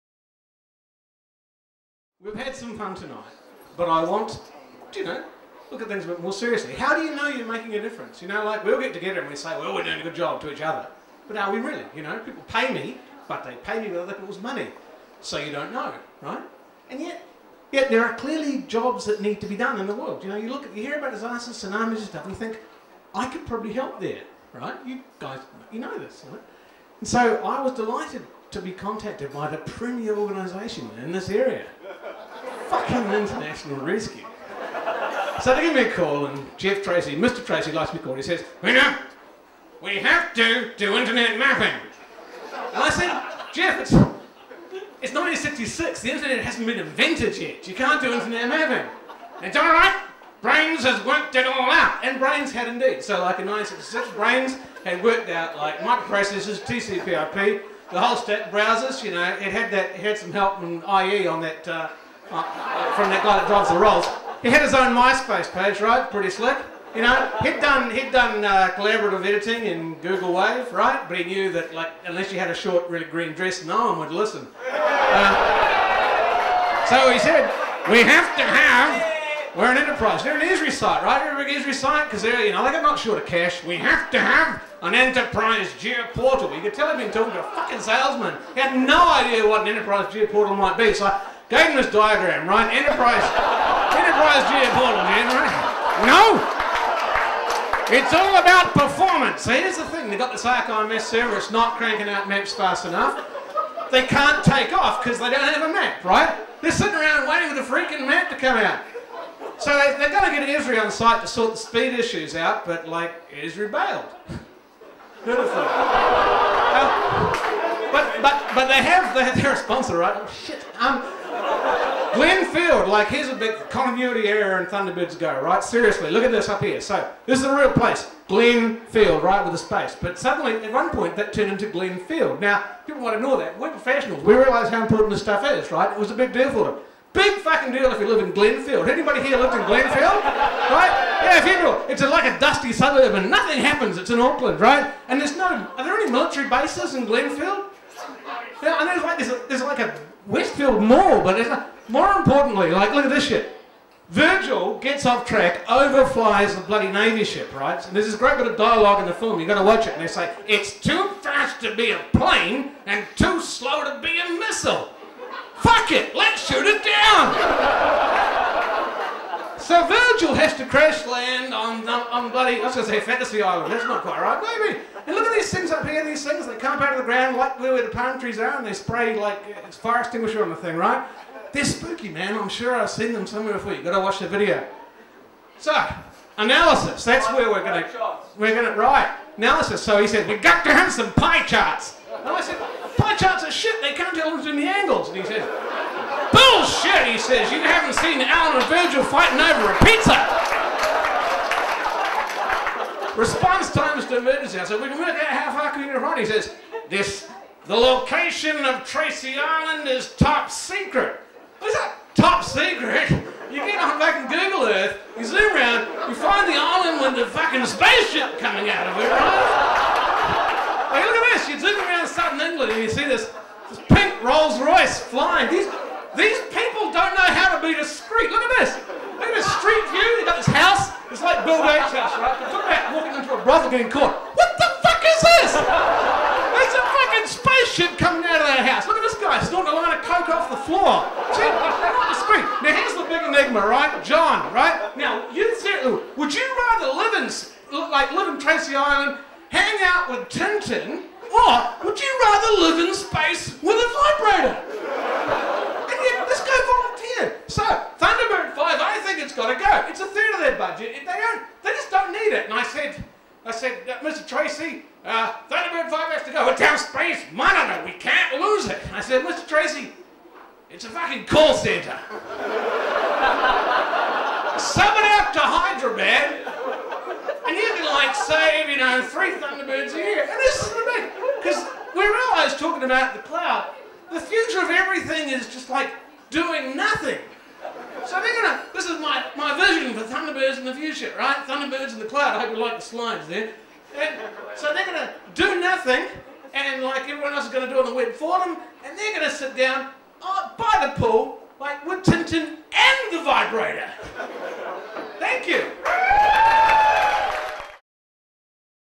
Peter Fraser - GIS for international rescue
Peter Fraser at Ignite Spatial, Sydney